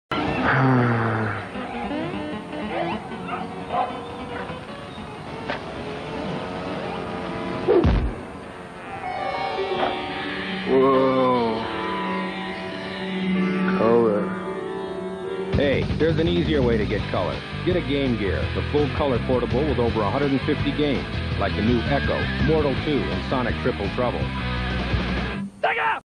Sega Game Gear vs. Nintendo Game Boy Advert
Sega comparing their Game Gear to Nintendo's Game Boy in a 90's advert.